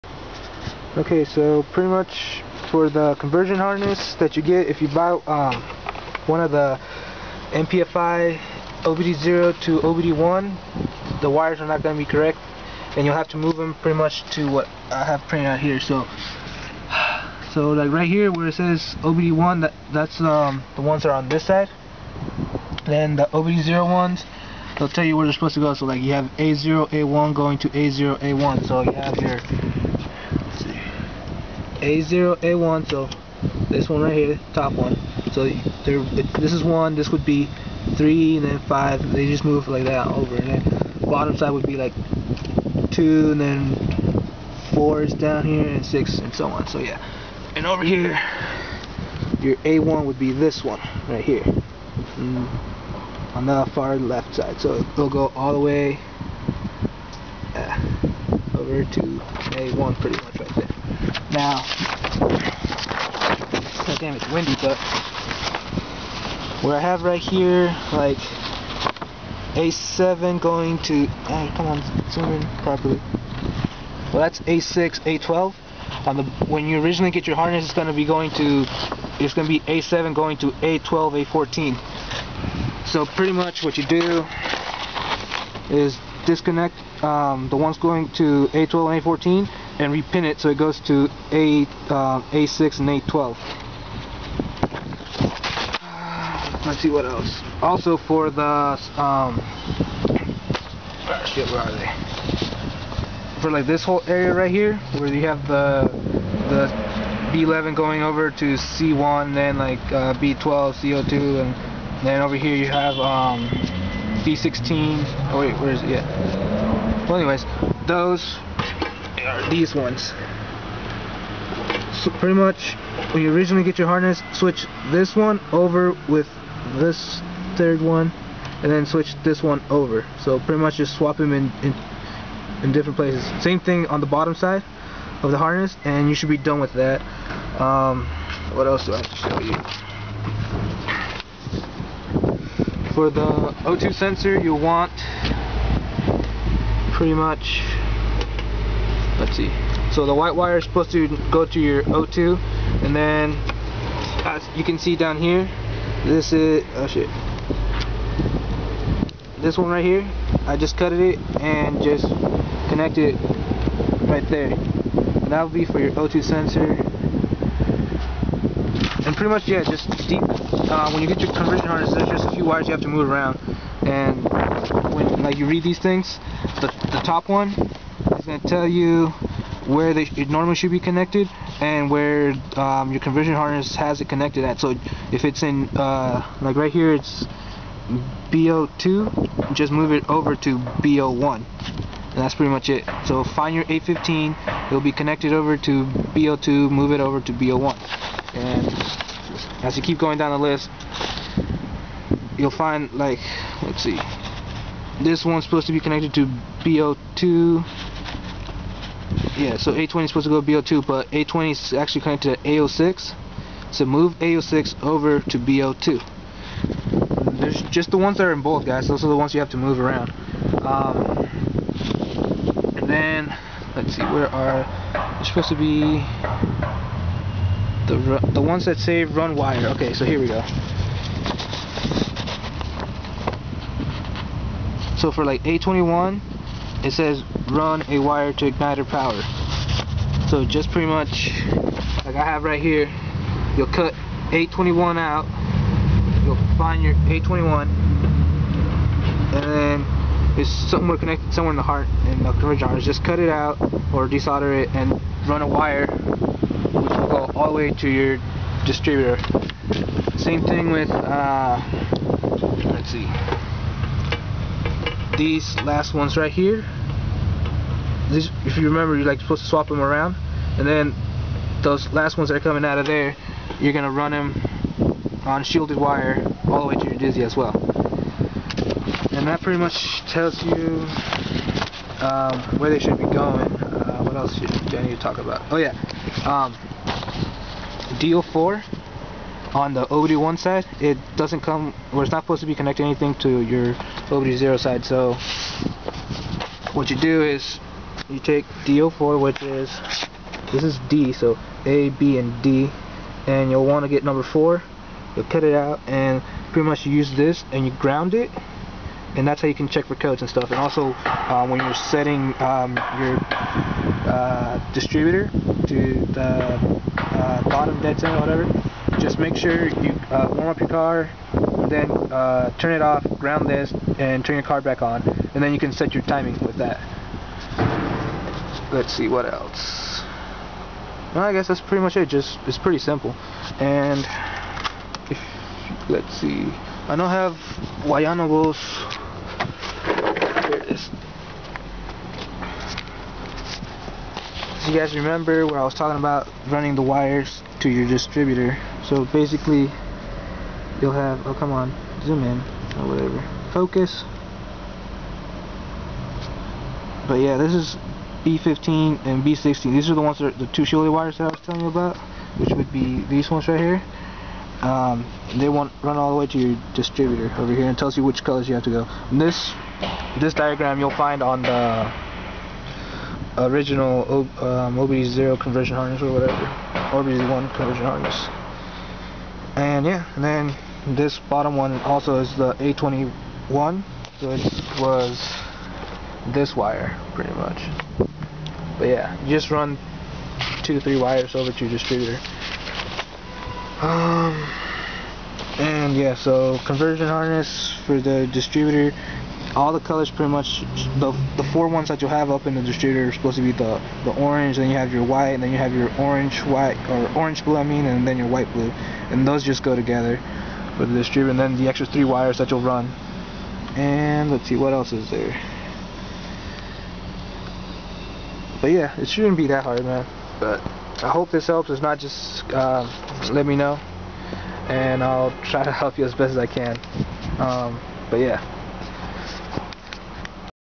OBD0 to OBD1 quick vid
Pin out for the OBD0 to OBD1 harness for the 88 accord i made from a civic OBD0 to OBD1 harness. This is to help fellow 3geez members with their conversions.